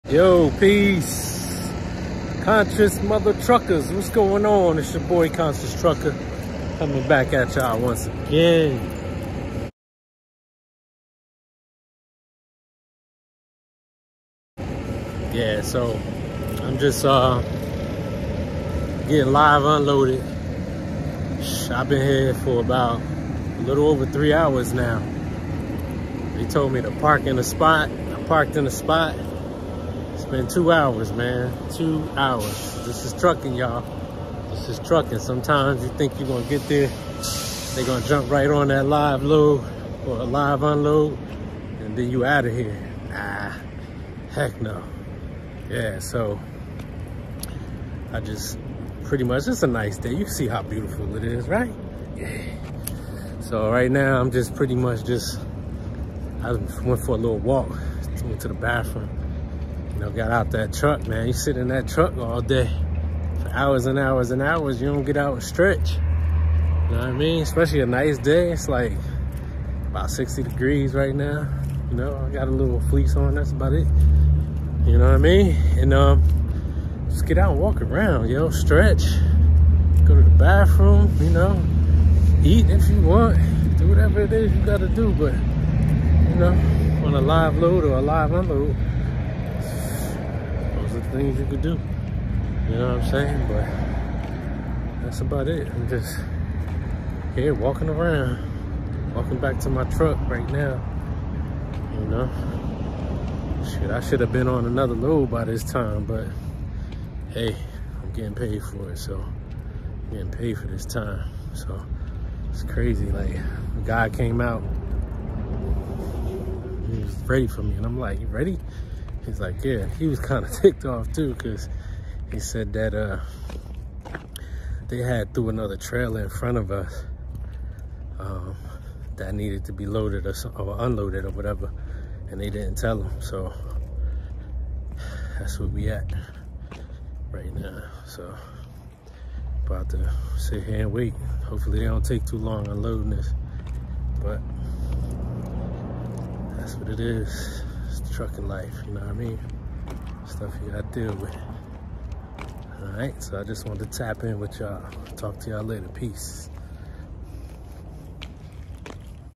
Live loads or live unloads vlog | Konscious Trucka
This day I waited a little over 3 hrs to be live unloaded. You never know how long you will be at a customer.